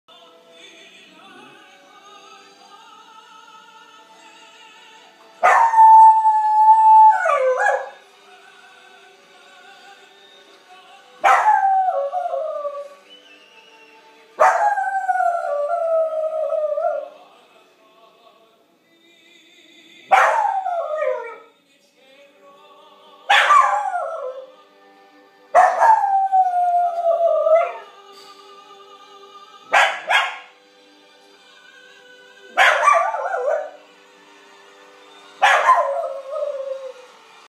Opera singing dog - Tibetan Terrier, Chloe
Our dog trying to sing along to opera its her favourite music!